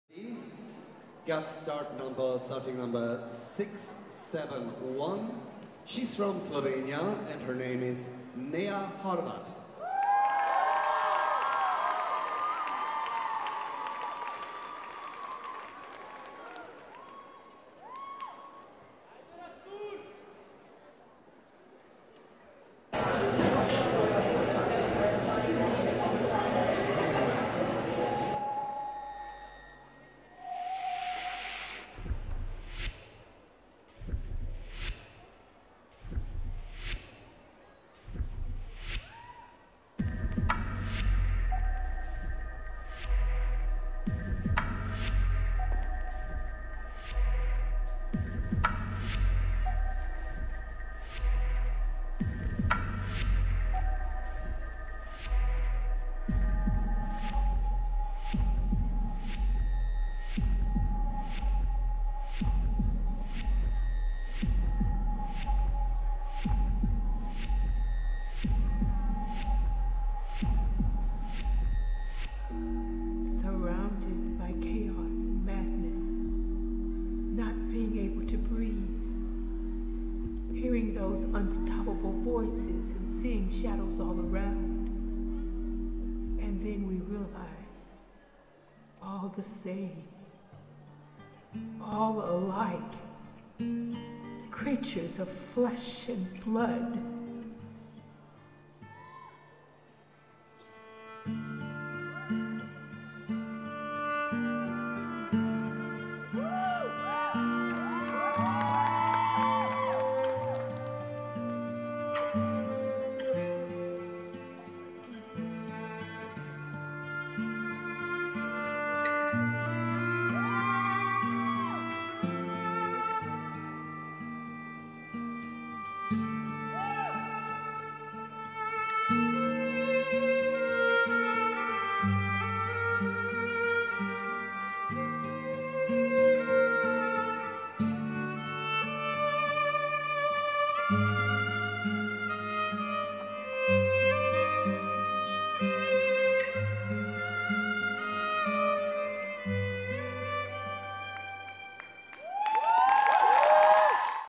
Flesh and blood - NEJA HORVAT modern solo junior | Jan Ravnik Choreography
1.place on World Championship in Wetzlar
Dancer: Neja Horvat
Choreography : Jan Ravnik